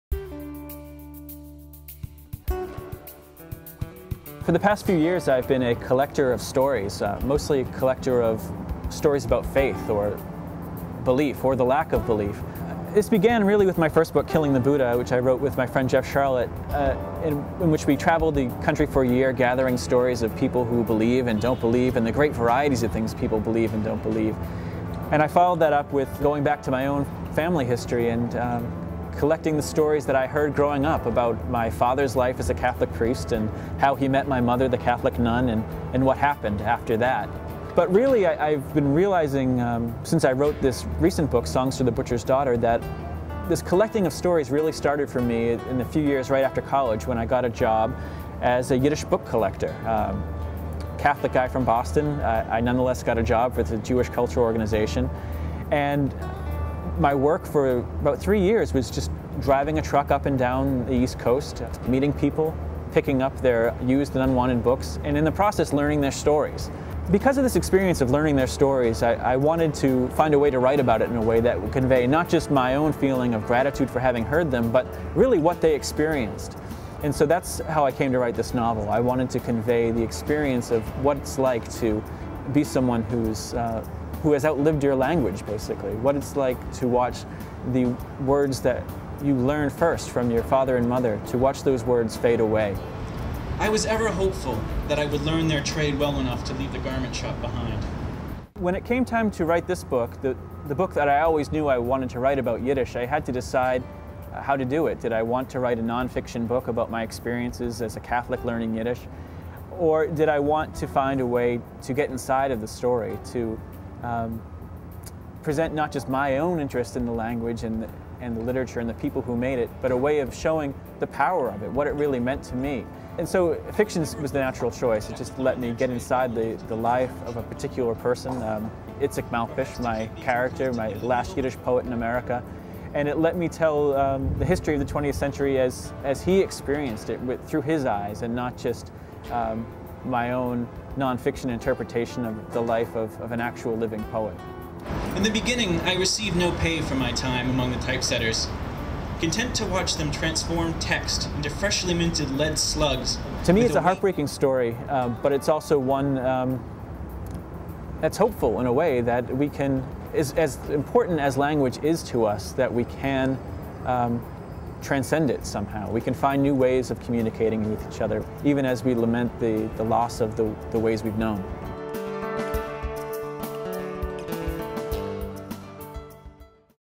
Peter Manseau - Songs for the Butcher's Daughter
Peter Manseau talks about the inspiration behind his book, SONGS FOR THE BUTCHER'S DAUGHTER. To learn more about this book, follow the